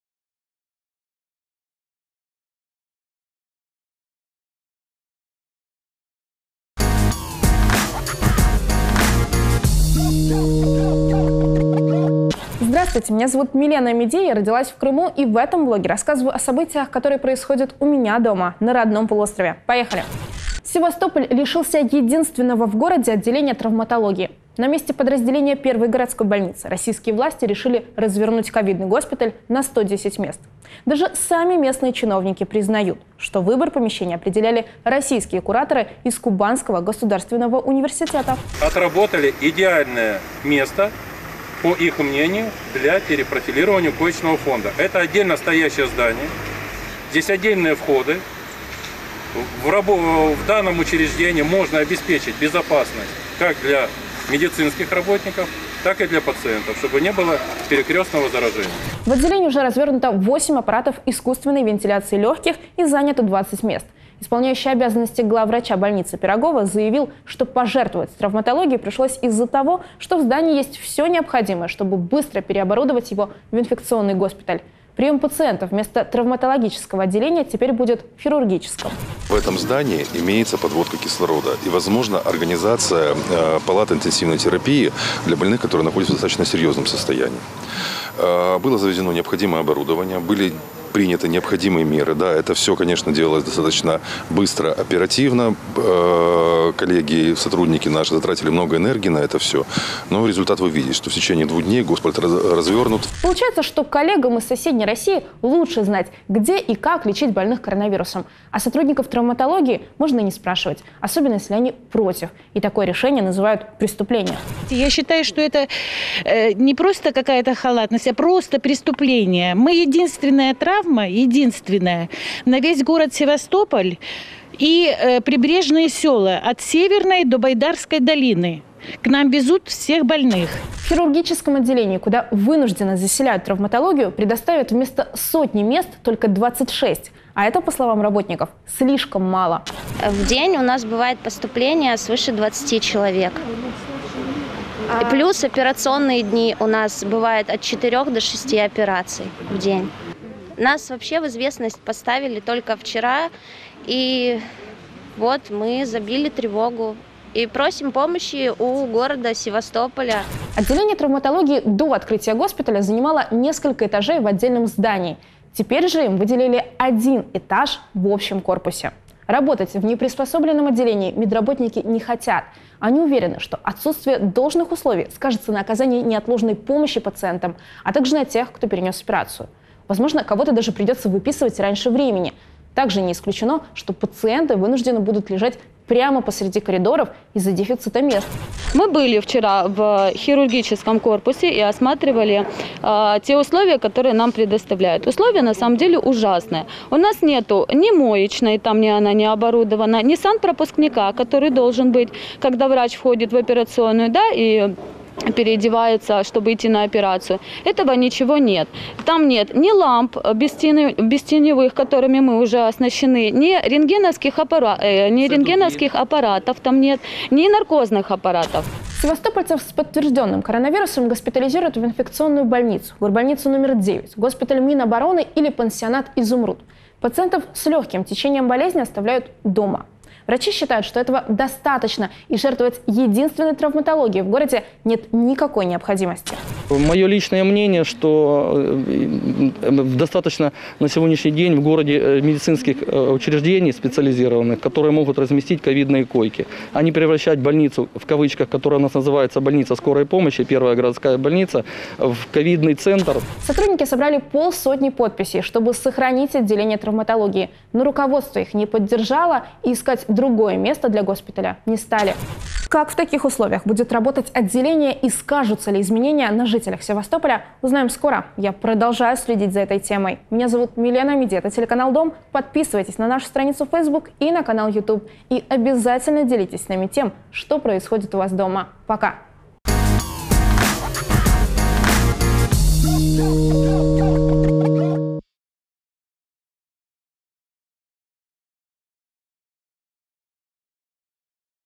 Медиков Севастополя выселили из отделения травматологии ради COVID-госпиталя
Севастополь лишился единственного в городе отделения травматологии. На его месте российские "власти" Крыма решили развернуть госпиталь на 110 коек для больных COVID-19. С сотрудниками травматологии даже не советовались о таком решении. Медики собрали подписи, чтобы их не закрывали, но руководство их не поддержало и не стало искать другое место для госпиталя.
Подробности – в блоге журналистки Милены Амеди.